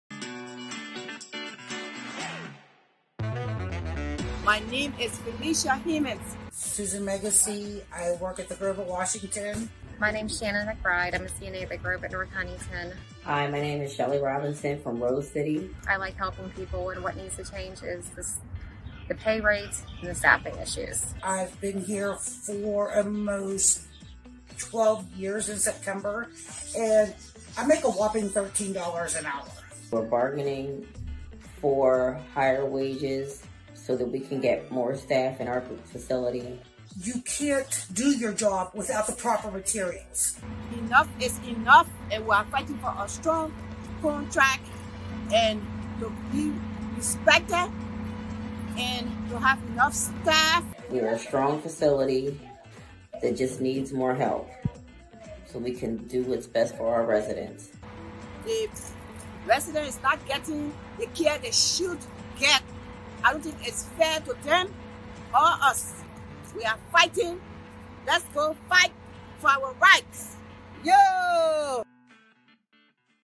4,500 nursing home workers are taking bold action for safe staffing in PA!
At 80 nursing homes across Pennsylvania, workers are taking action NOW for better staffing and strong contracts that protect residents and workers.

It all starts May 25th: Caregivers at 41 nursing homes are rallying for safe staffing. Send a message to the workers on the picket line here: betterstaffingnow.com